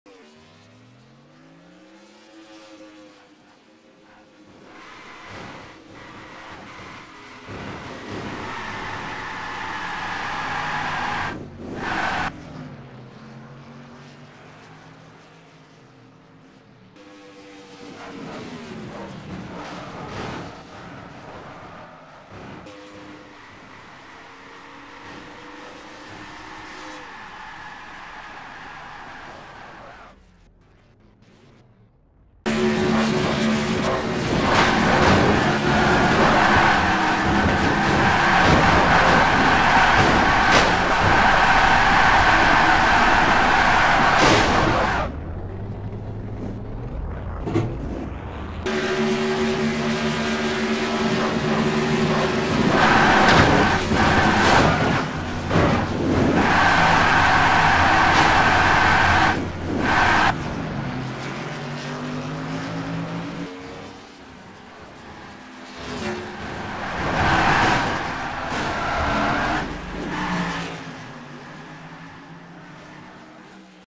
Pack Racing At Talladega Gone Wrong
Mirror driving will bite you as it did me